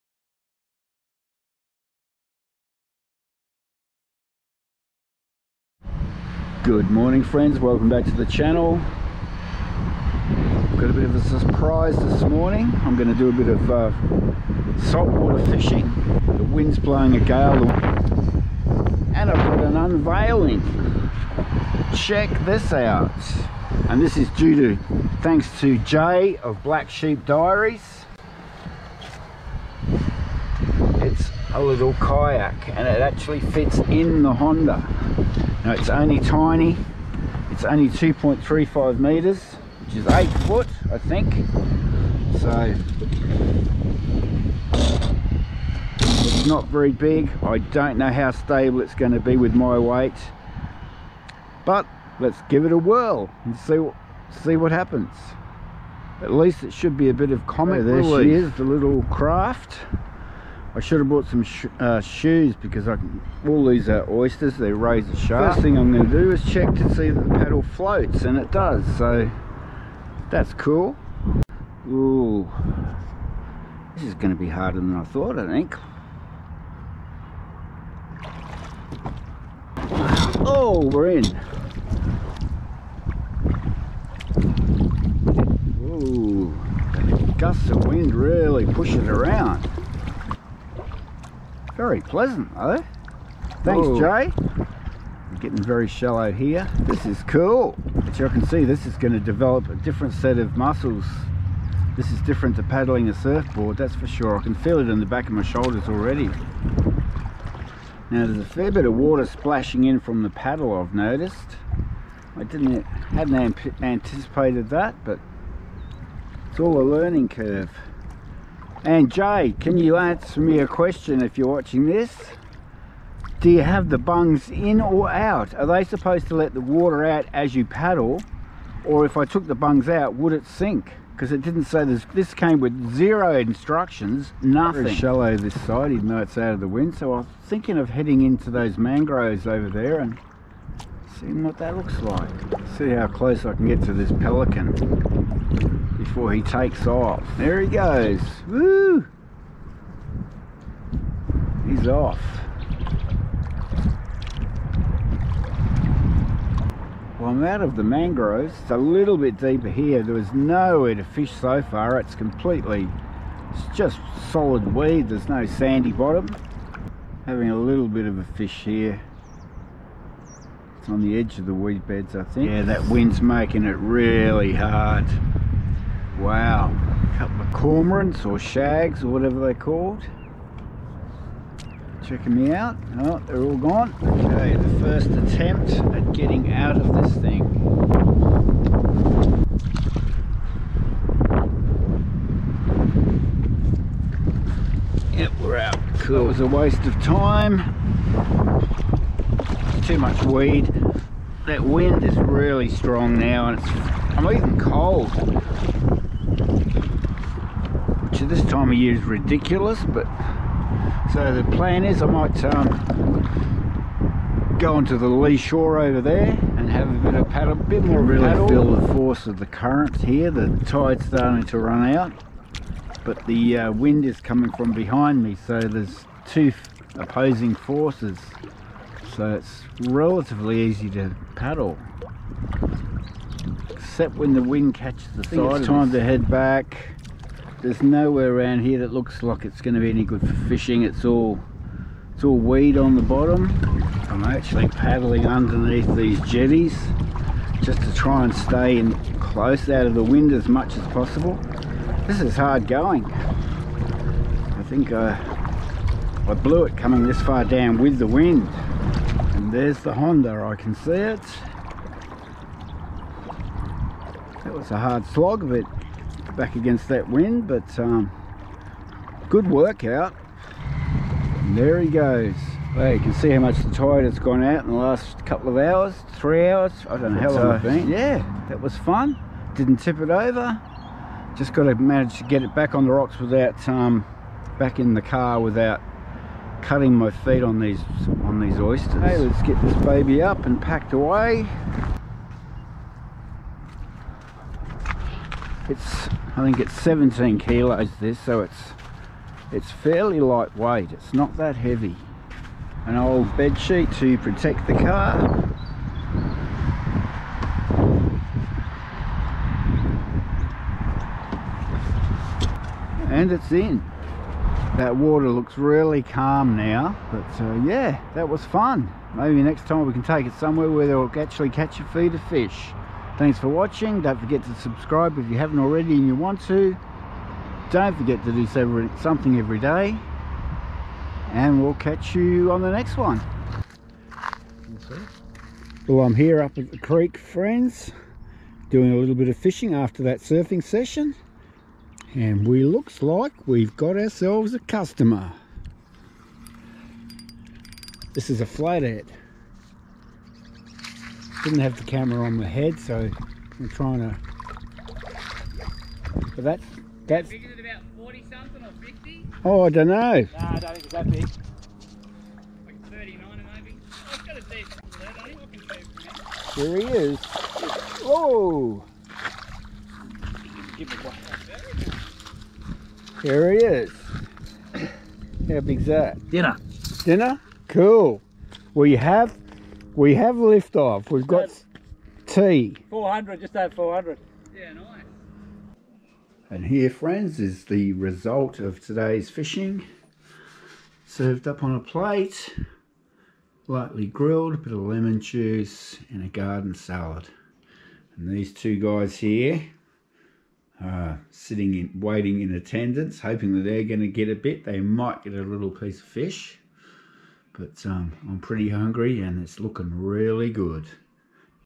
THE WORLDS SMALLEST KAYAK!!! PRYML SPARTAN 2.35M - WHAT COULD POSSIBLY GO WRONG... :)
At 87kg, + seat (3kg) paddle, fishing gear & Thermos of tea, I'm perilously close to the 100kg weight limit for this craft! First ever attempt at kayaking!